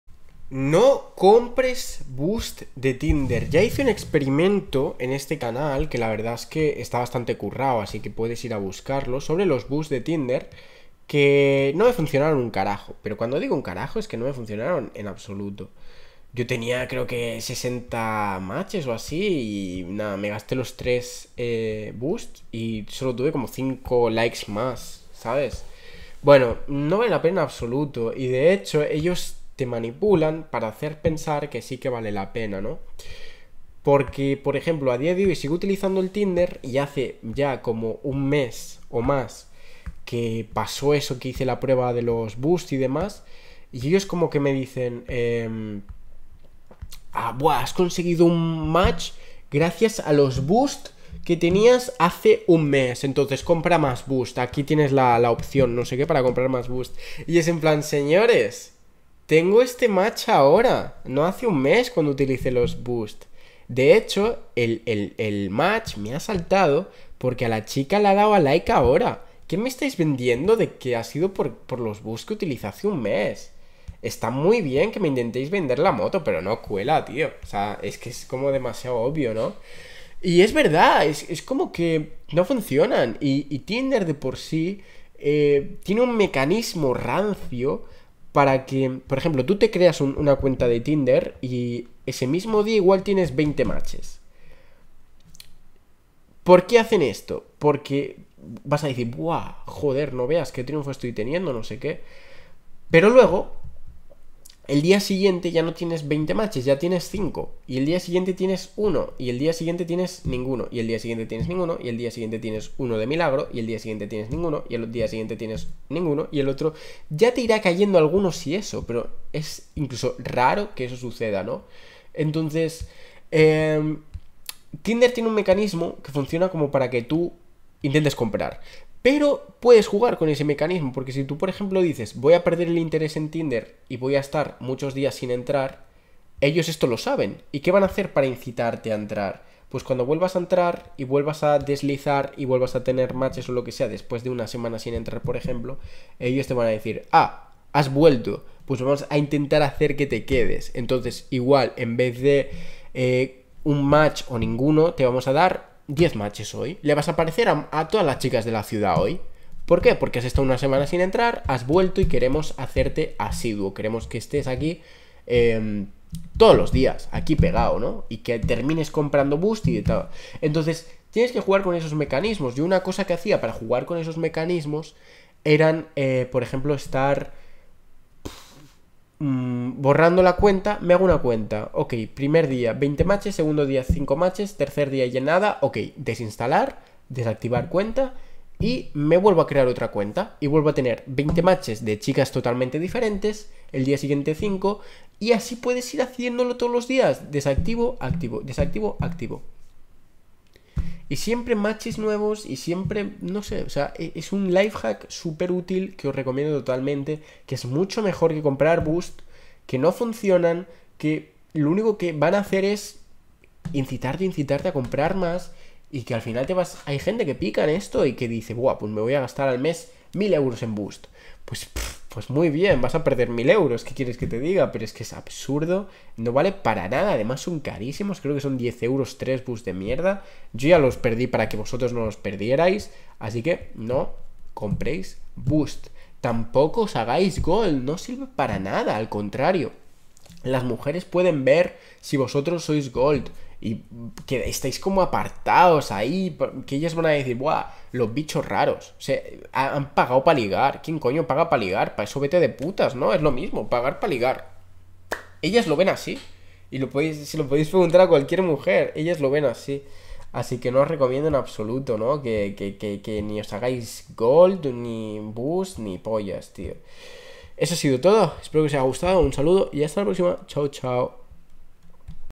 NO compres BOOSTS de TINDER
Os cuento mi resumen y conclusión tras probar los boosts de Tinder.